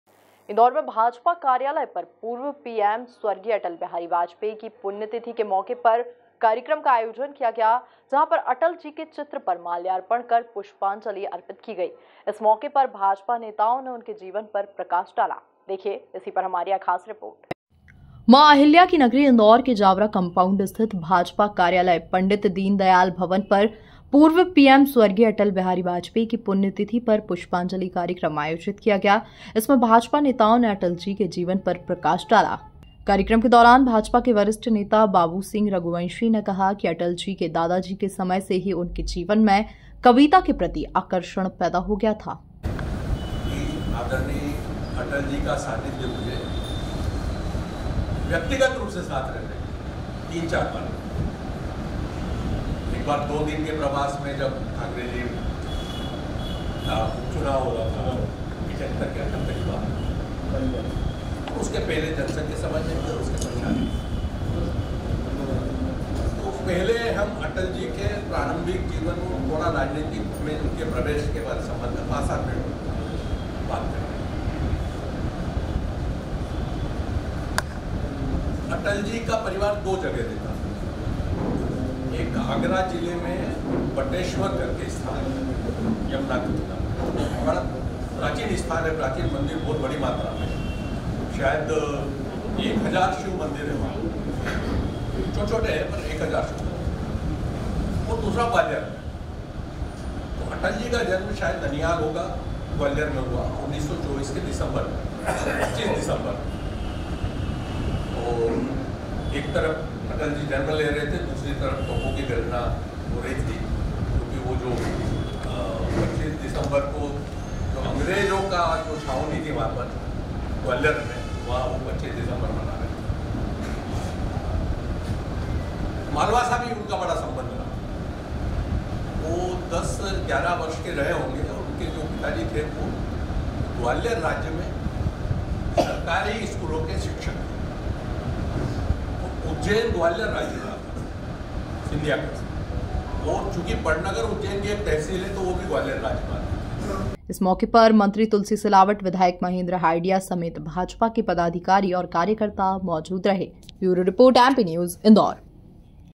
भाजपा नेताओं ने स्व. अटल बिहारी वाजपेयी को किया याद, मंत्री और नेताओं ने दी श्रद्धांजलि! MP News
भाजपा नेताओं ने स्व. अटल बिहारी वाजपेयी को किया याद, मंत्री और नेताओं ने दी श्रद्धांजलि! MP News Indore